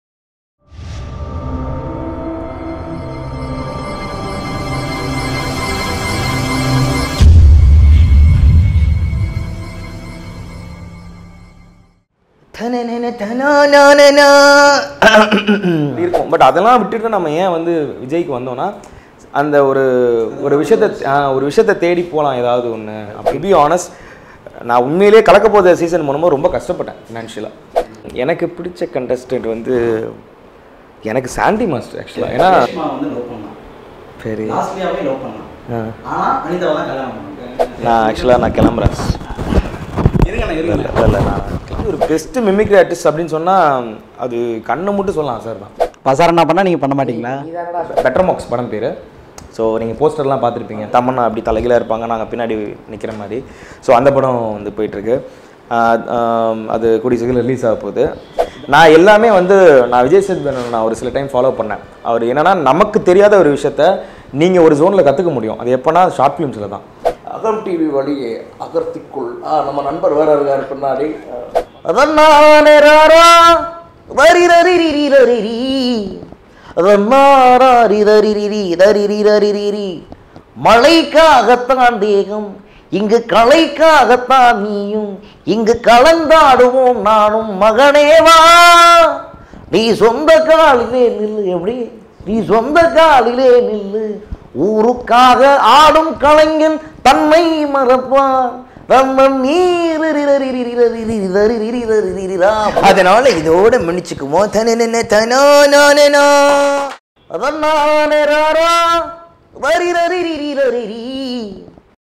TSK (Trichy Saravanan Kumar) Interview || Celebrities Talk Show || Promo || Karuppu Koda
A promo Video of our Upcoming Celebrities Talk Show

An Talk Show About TSK aka Trichy Saravanan Kumar

Karuppu Koda (Black Umbrella) is a bunch of Fresh and Young Talented Creative People which aims to Entertain an audiences.